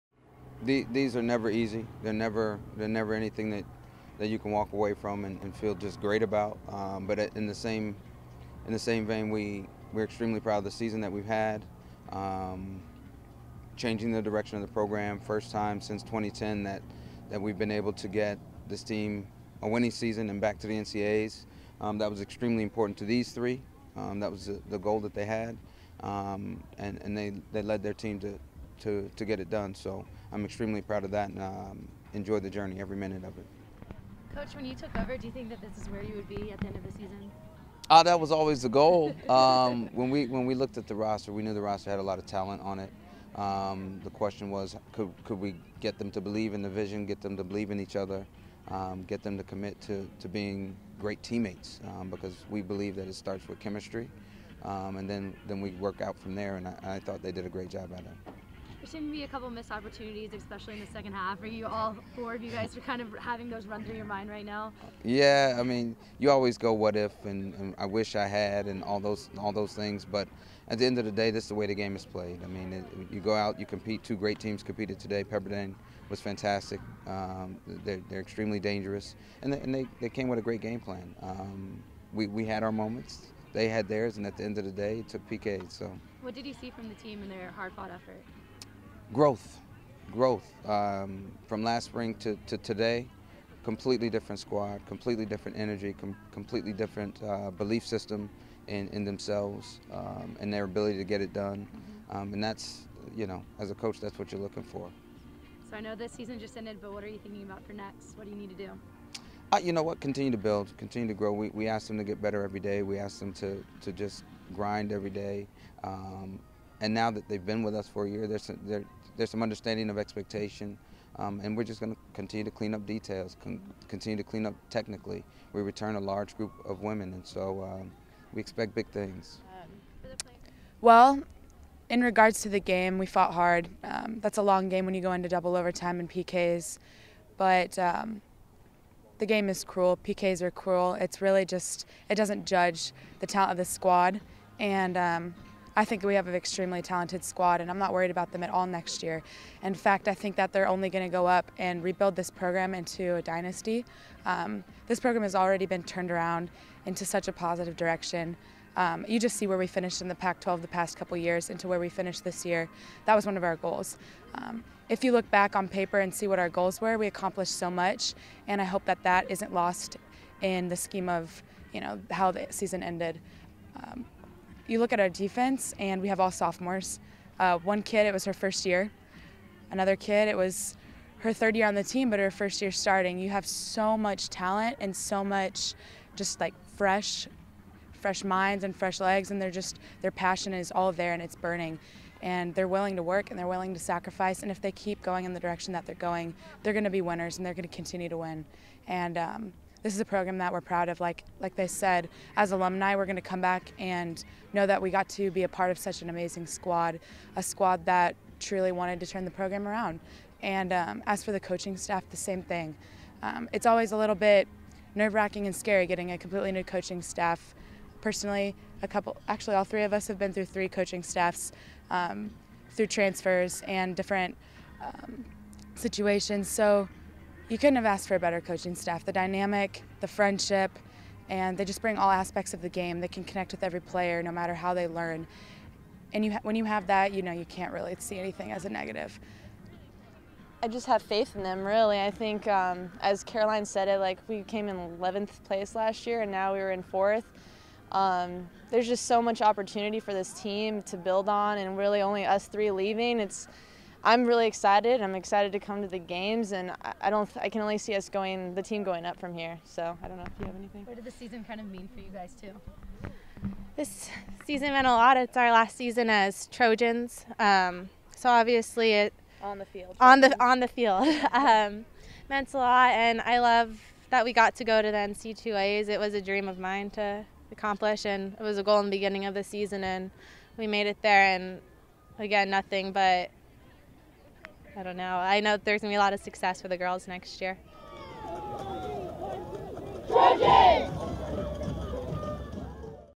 Women's Soccer - USC-Pepperdine NCAA Round 1 Recap
USC dropped it's NCAA Tournament Round 1 matchup with Pepperdine in free kicks on Saturday in Malibu. Even 1-1 through the 90 minute match and 20 minutes of overtime, the Trojans fell 4-2 in free kicks.

Hear from Keidane McAlpine as he reflects on a successful first season as Head Coach, and Seniors Caroline Stanley (GK), Alex Quincey (F), and Jessica Musmanno (F) discuss the game and the direction of the program.